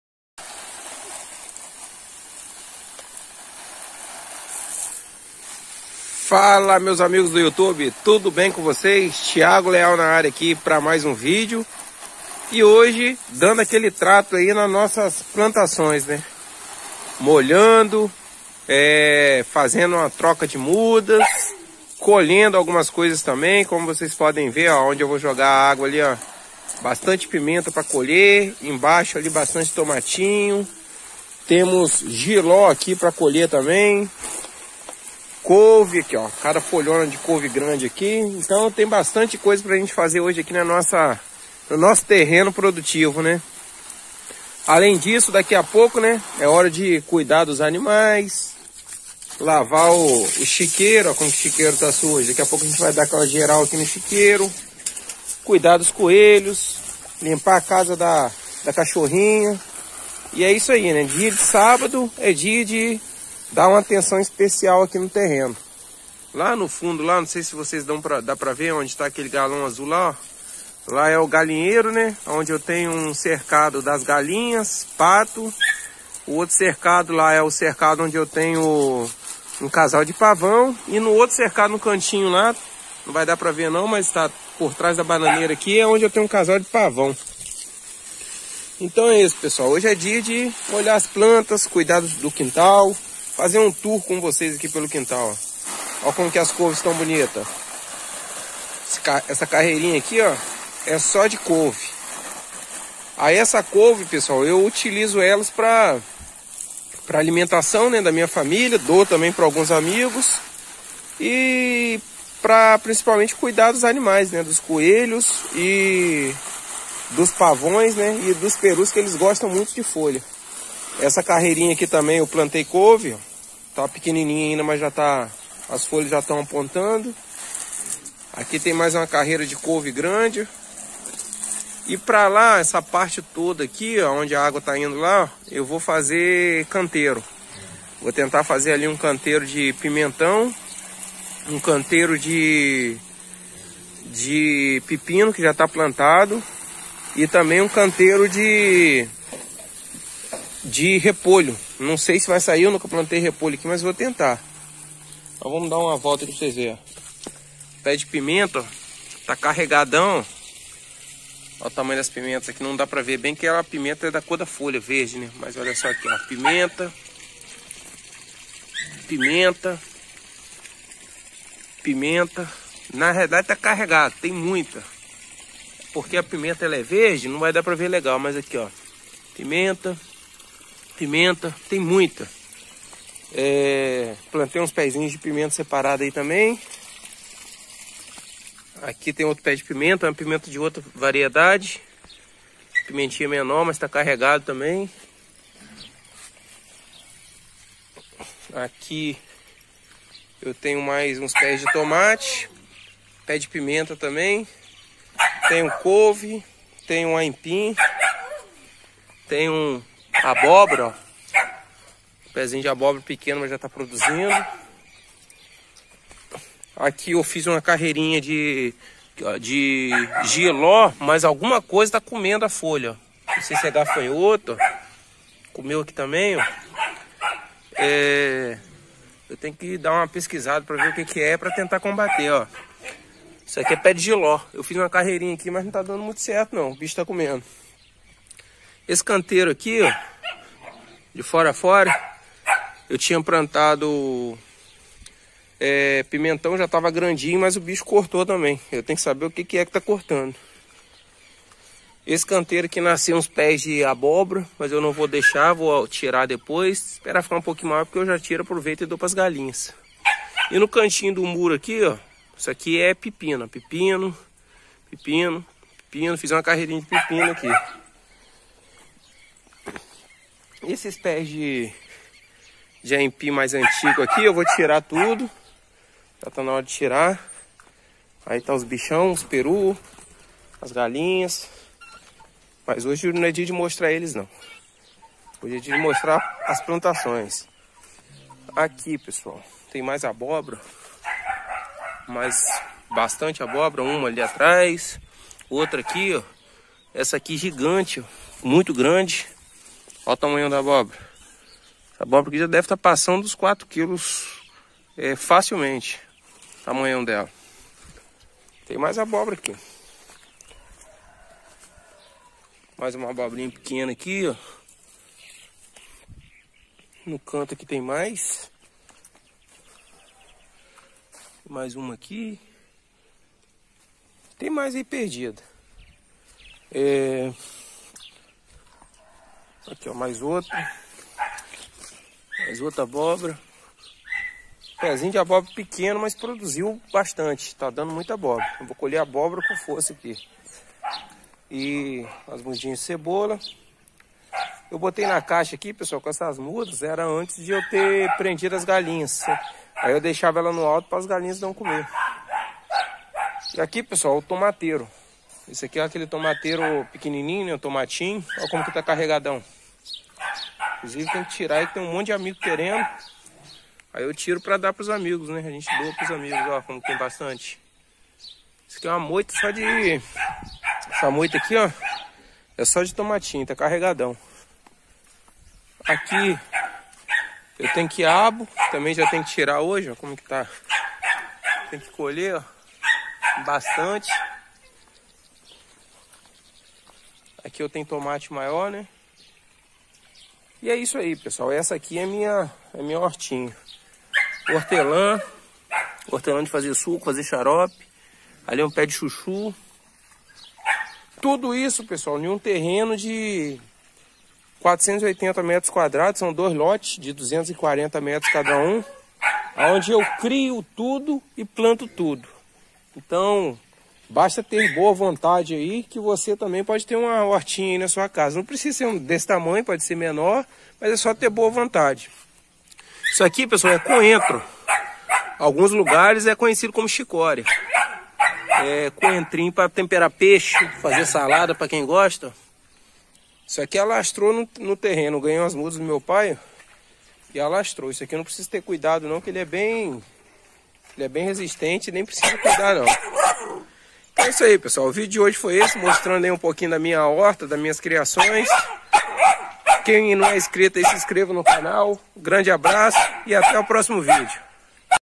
MOLHANDO A HORTA e MOSTRANDO AS PLANTAS.
Olá meus amigos leais, nesse vídeo eu vou mostrar um pouco da minha horta, minhas variedades de plantas, meus canteiros e a forma que eu molho os canteiros.
Aqui no terreno além da horta eu tenho criação de pequenos animais.

Tiago Leal